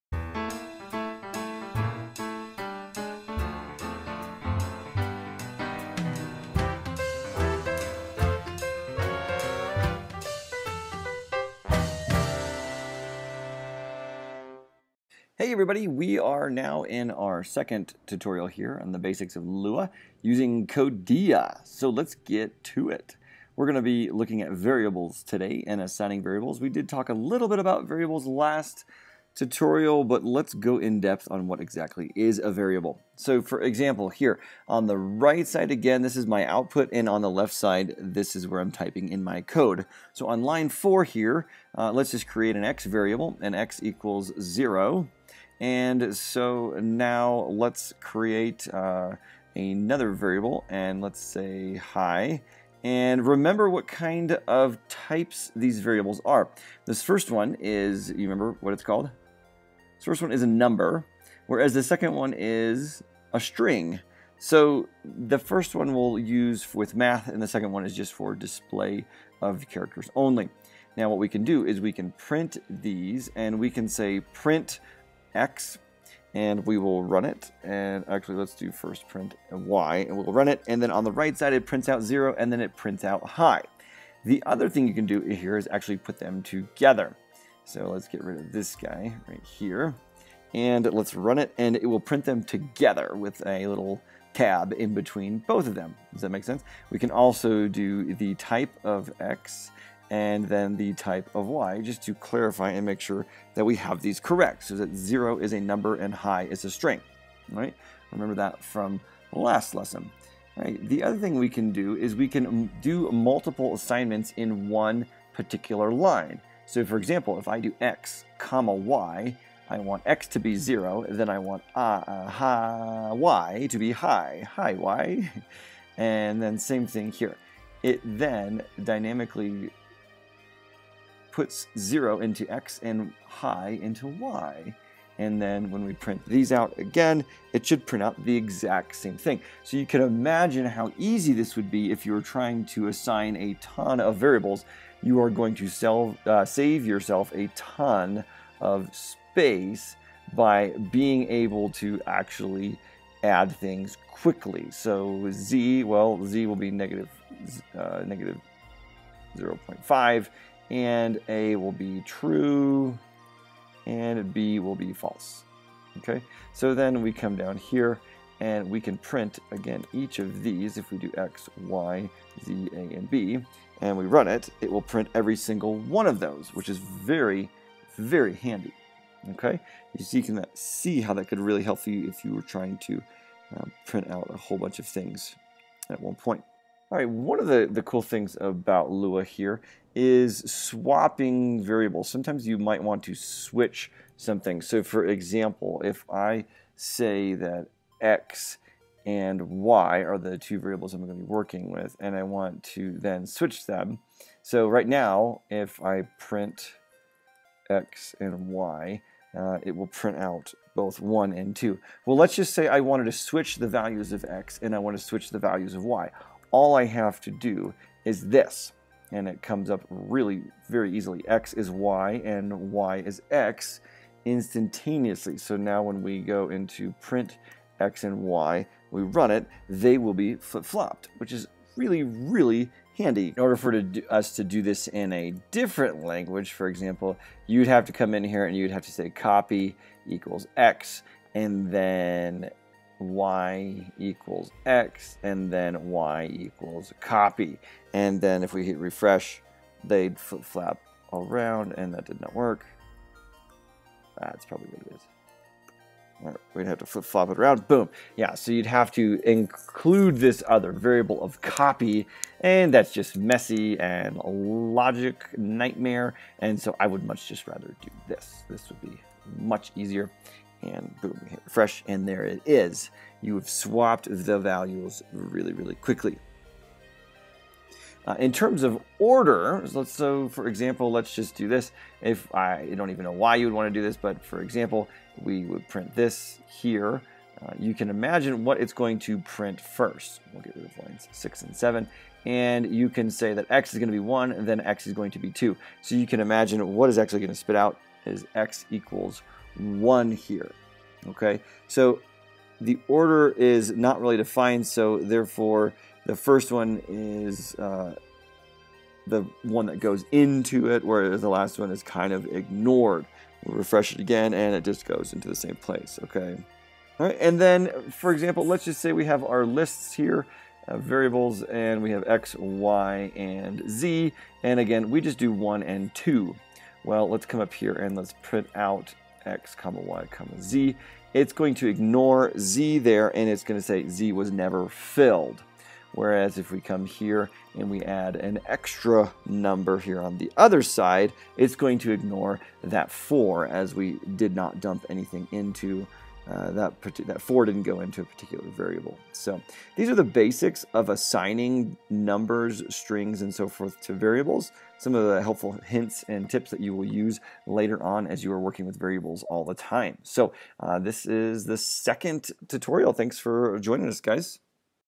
Codea Tutorials #002--Variables in Lua, iPad Programming
Here's a short tutorial on variables in Lua on the iPad.